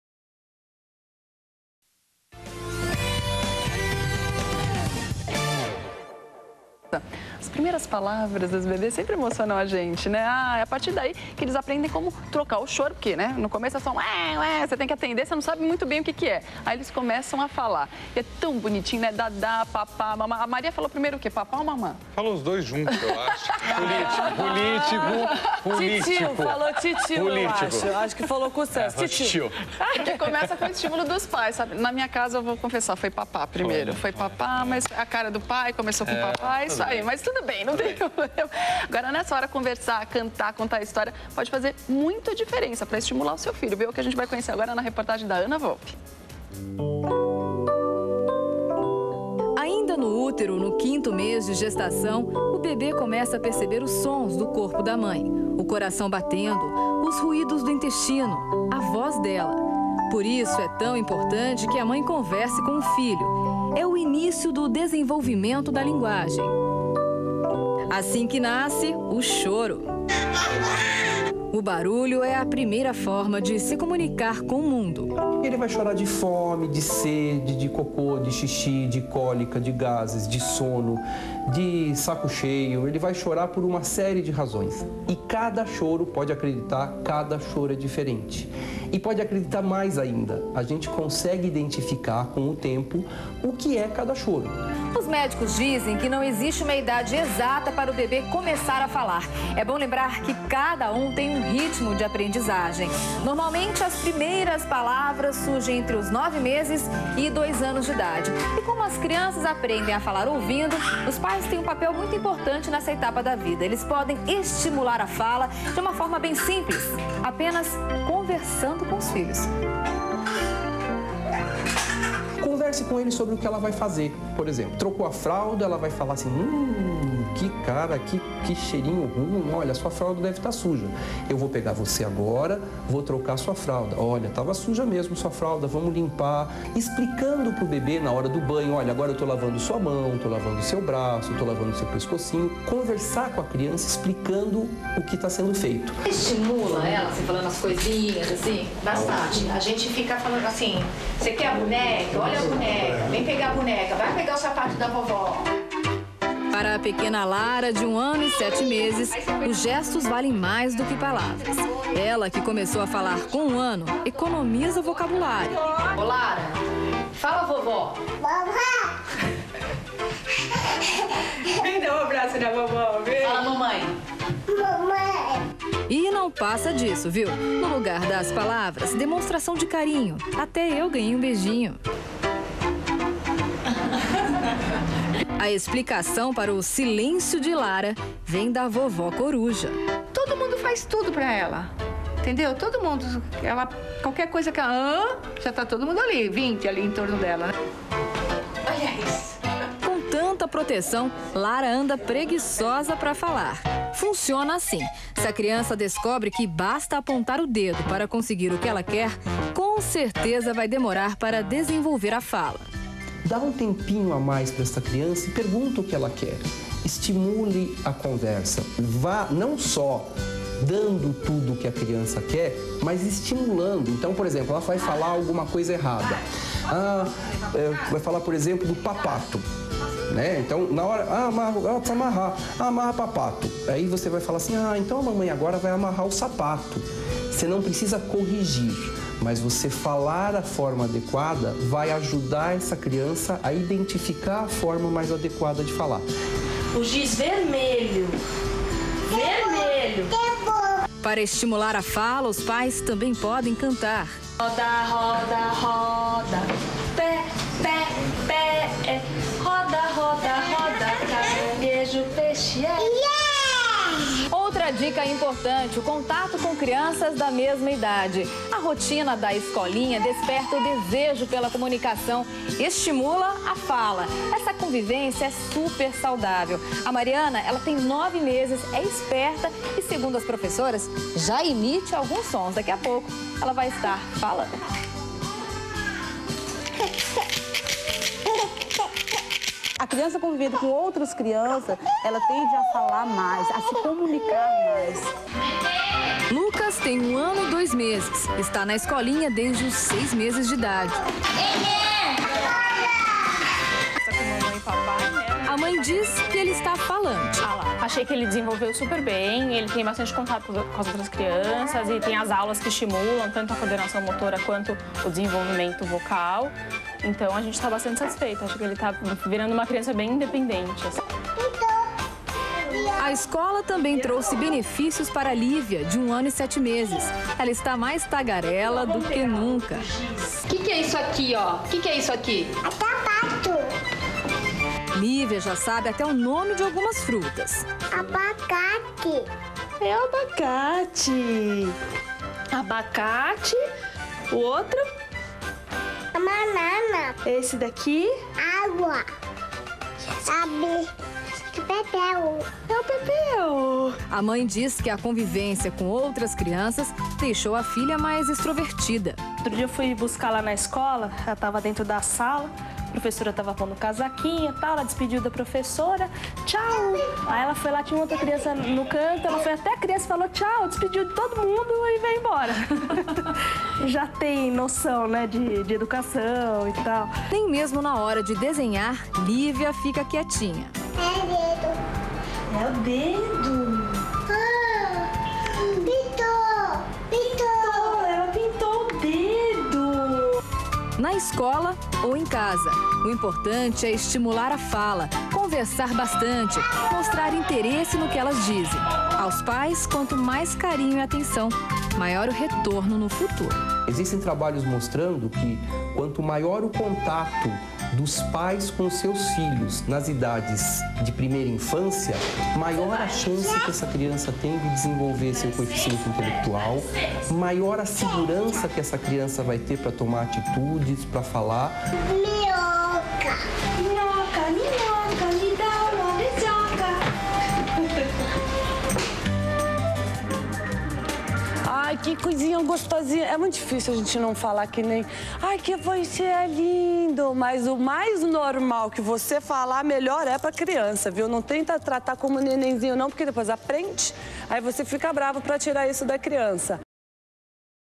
PEDIATRIA - Como estimular o bebê a falar?
Para aprender a falar é preciso que algumas habilidades auditivas estejam prontas e que a musculatura responsável pela articulação dos sons da fala também esteja suficientemente desenvolvida e estimulada. Por isso, fale com seu bebê enquanto cuida dele, cante para ele as cantigas de ninar, aconchegue-o com o calor de seu abraço.
Cuide para que ele possa mamar no peito, pois os músculos da boca e da face serão estimulados adequadamente. Quer saber mais? Acesse